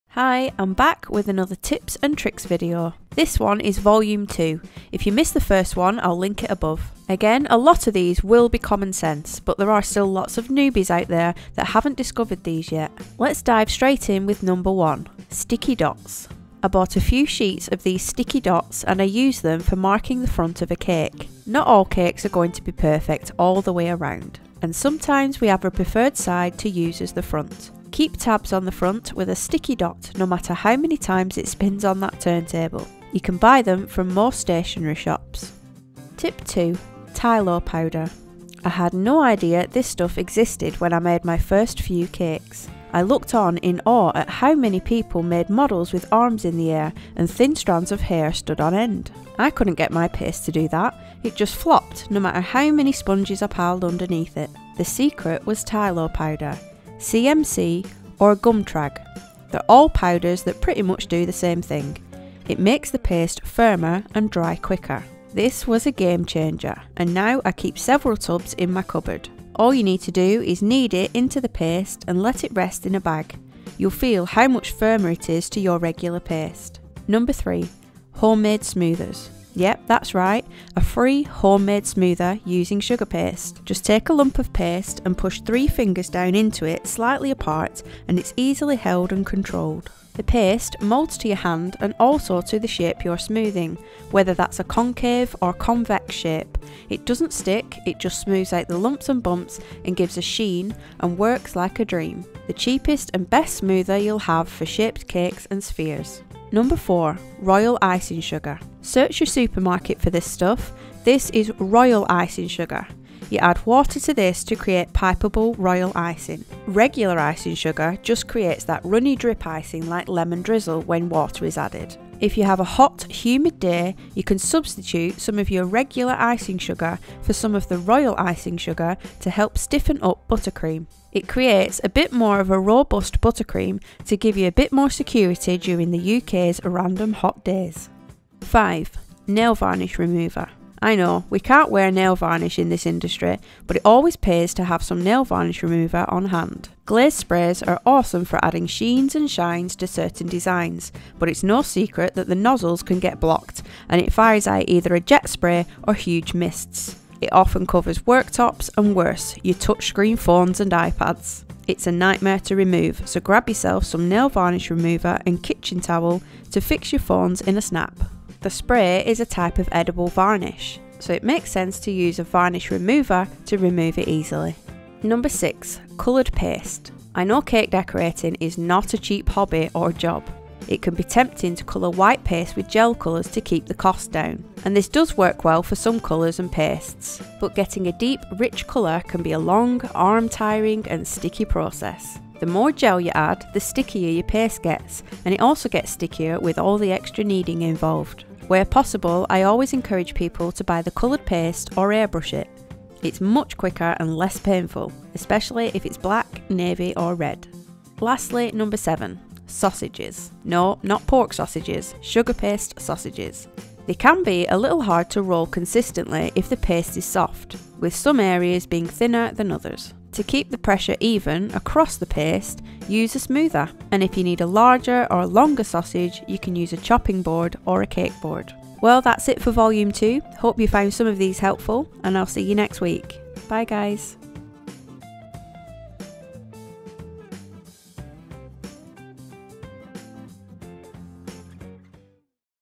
Cake Decorating Tips & Tricks for Beginners Volume 2 | Cherry Basics
Hi! Video containing Tips and Tricks for Cake Decorators, hope you find them useful!

►MENTIONED◄
Tylo Powder
Royal Icing Sugar
Nail Varnish Remover
Glaze Spray
Sticky Dots
Kroma Airbrush Colours